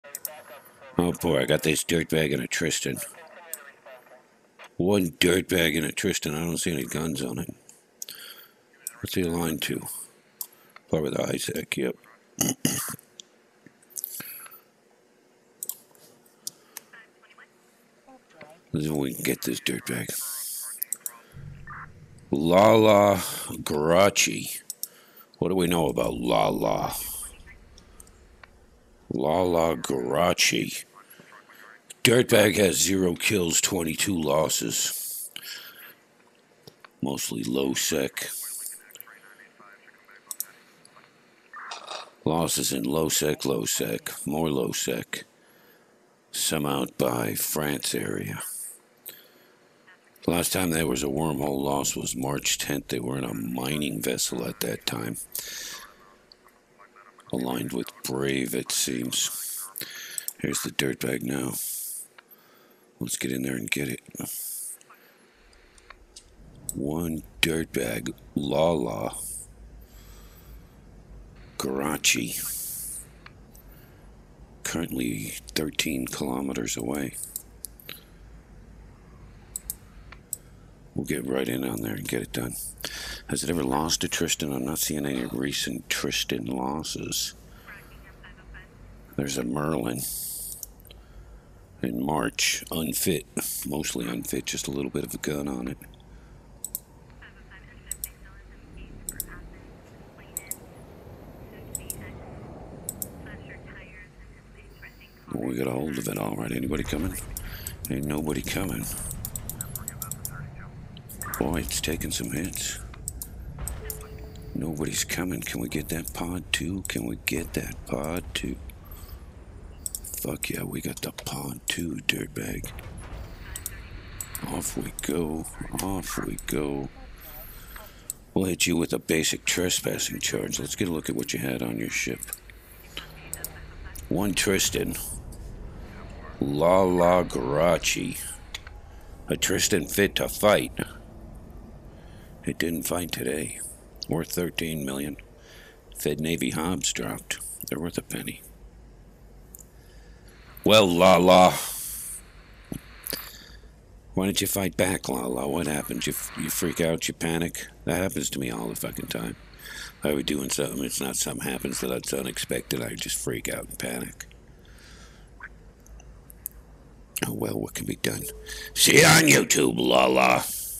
Lala Guaraci - Forgot to fight back
I'm an old man and I'm just trying to take a nap but these dirtbags keep criming away keeping me up afternoons and making me work overtime! This particular dirtbag forgot to fight back! Not Brave!